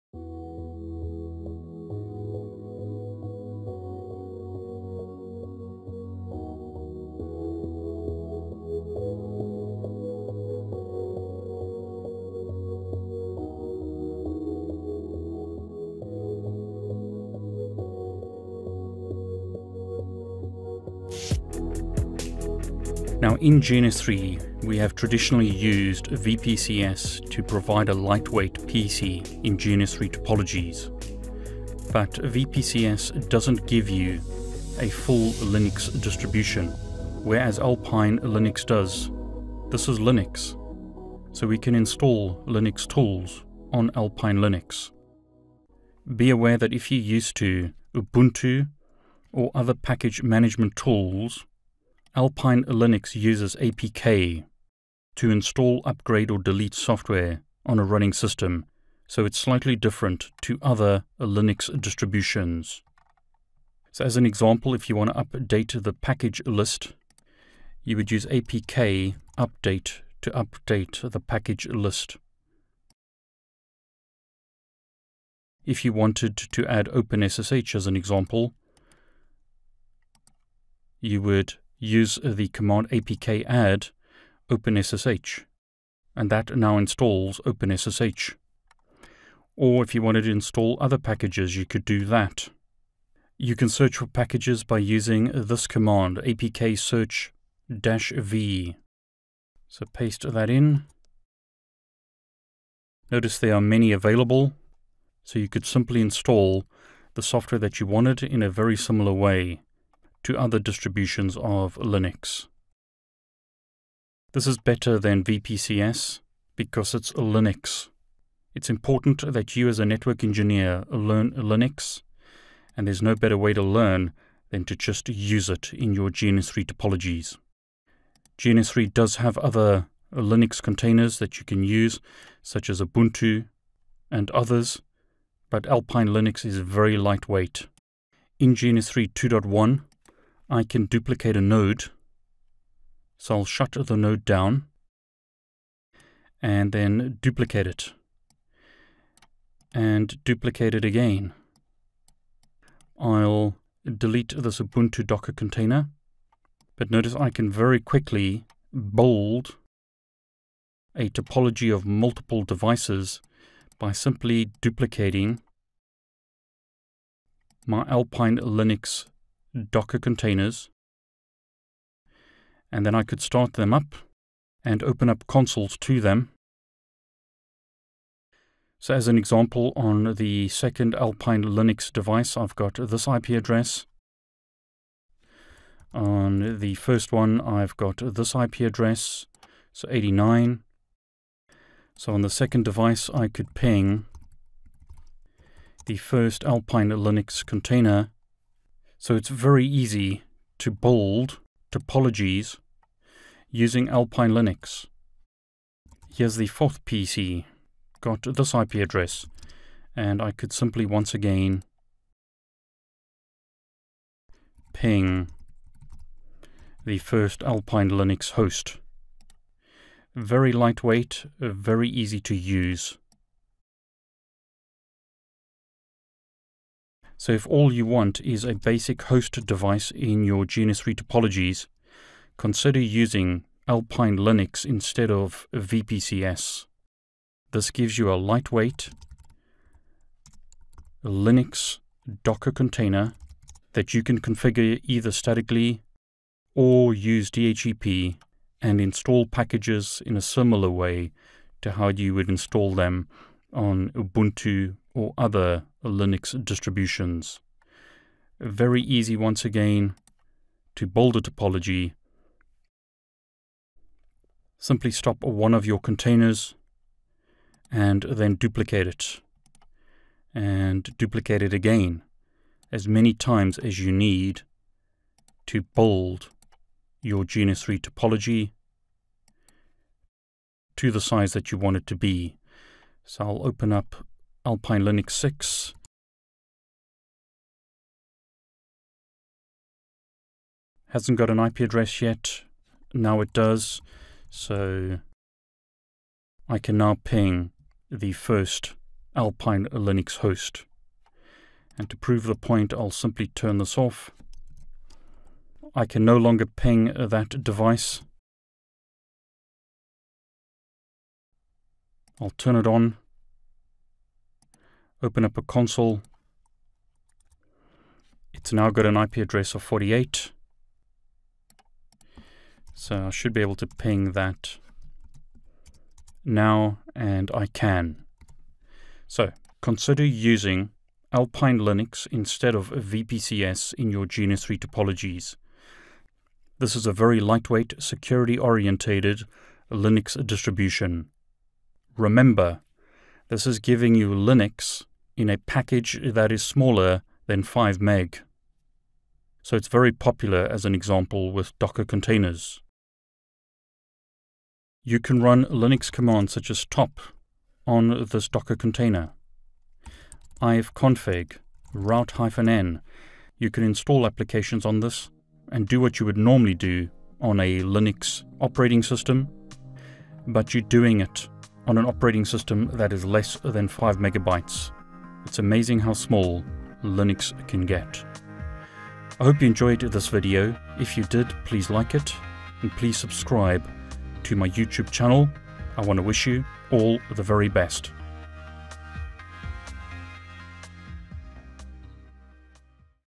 GNS3 Talks: Alpine Linux - replace VPCS in your GNS3 topologies (Part 2)
A lot of people use VPCS in GNS3 topologies. A better way to do this is to use Alpine Linux because you get a lightweight Linux distribution to use in your GNS3 topologies.